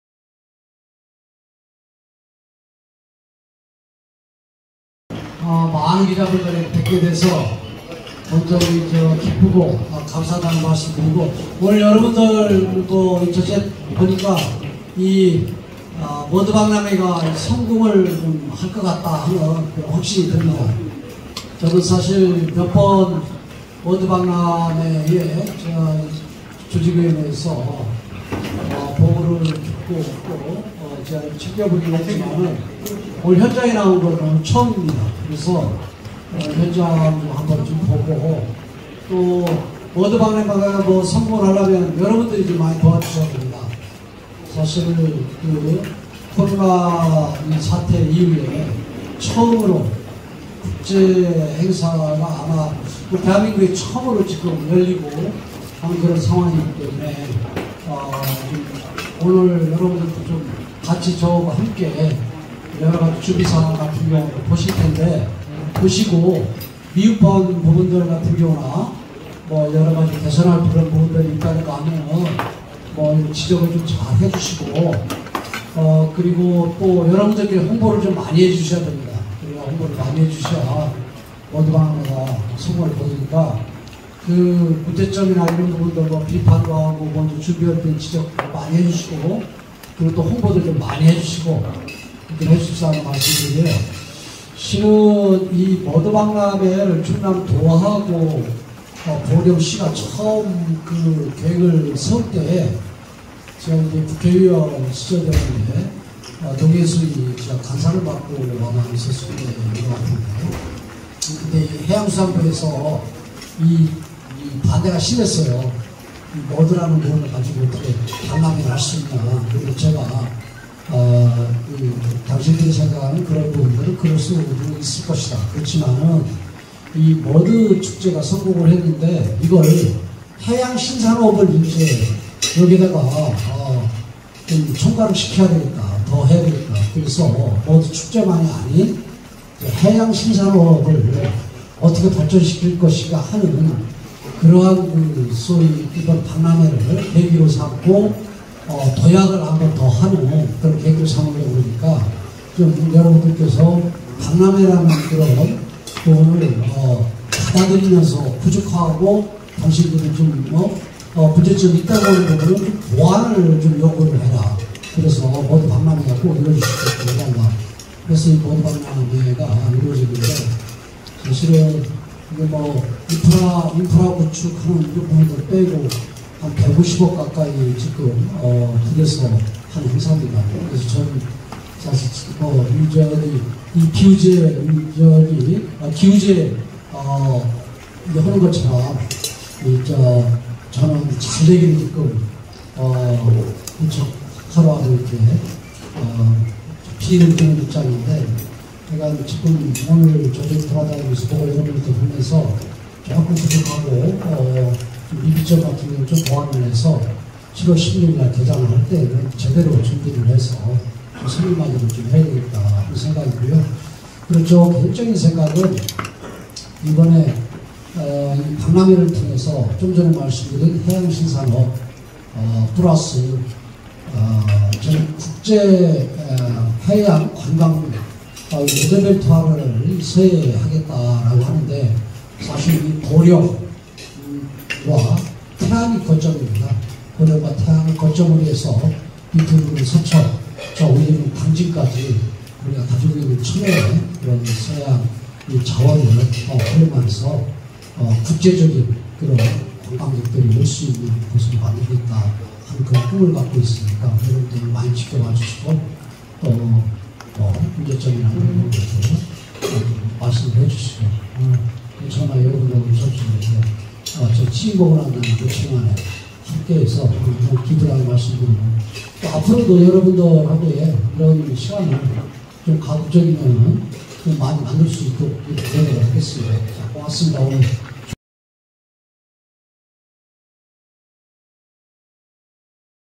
(OTN뉴스)김태흠 충남도지사 기자간담회, '해양의 재발견, 머드의 미래가치' 보령해양머드박람회 홍보
김태흠 충남도지사 기자간담회,  '해양의 재발견, 머드의 미래가치' 보령해양머드박람회 홍보.